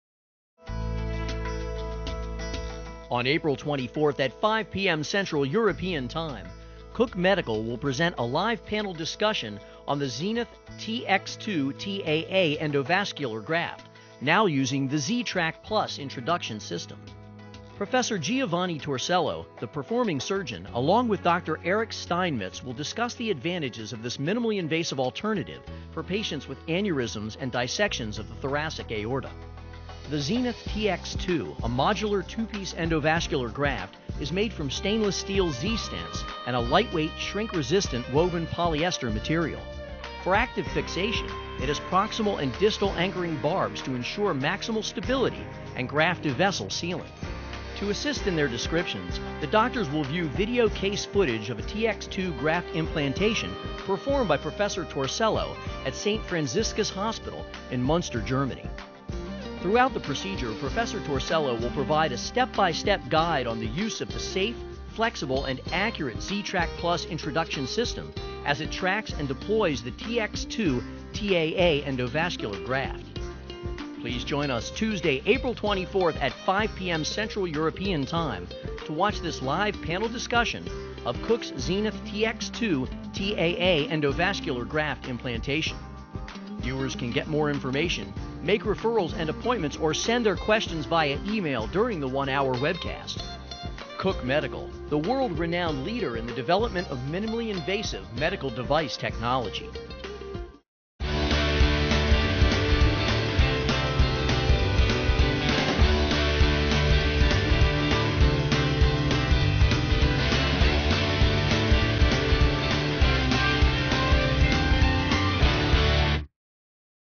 Live Webcast: Thoracic Aortic Aneurysm Repair w/ Cook Zenith
Surgeons from St. Franziskus-Hospital in Muenster, Germany will demonstrate the repair of a thoracic aortic aneurysm (TAA) with the Cook Zenith TX2® Endovascular Graft in a live webcast on OR-Live.com The webcast, featuring the TX2's new Z-Trak Plus Introduction System, will be broadcast Tuesday, April 24th at 5 p.m. Central European time.
Prof. Giovanni Torsello, Chief of the Department of Vascular Surgery at St. Franziskus-Hospital, Muenster, Germany, will discuss his performance of the case along with an international panel of European doctors who are experts in this device technology. Viewers will have the ability to email questions for inclusion in the interactive presentation.